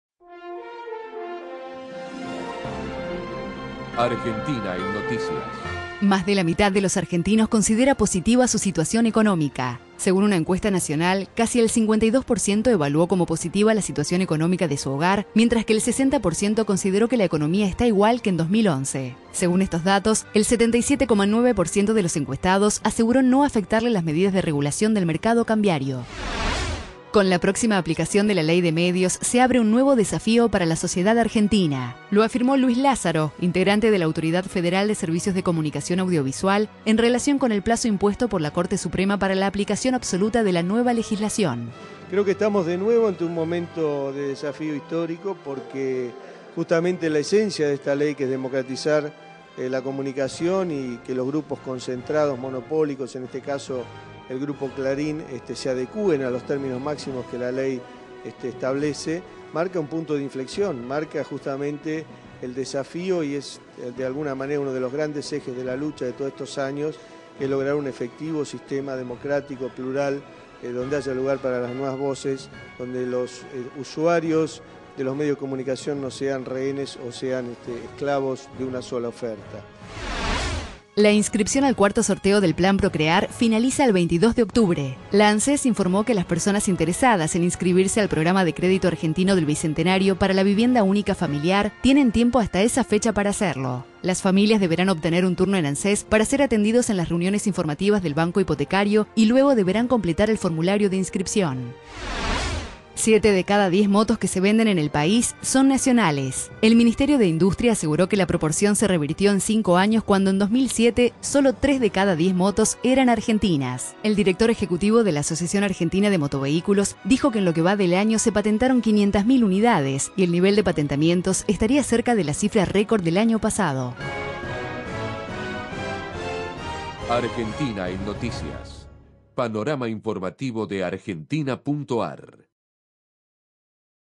AEN 03-10-2012 18HS: Economía satisfactoria_ 7D Ley de Medios_Inscripción 4ºsorteo PROCREAR
Más de la mitad de los argentinos está satisfecho con su situación económica
Luis Lázzaro sostuvo desde el AFSCA que la próxima aplicación absoluta de la Ley de Medios abre un nuevo desafío para la sociedad argentina
El próximo 22 de octubre concluye el plazo para la inscripción al cuarto sorteo de los planes ProCreAr
Siete de cada 10 motos que se venden en la Argentina son de fabricación nacional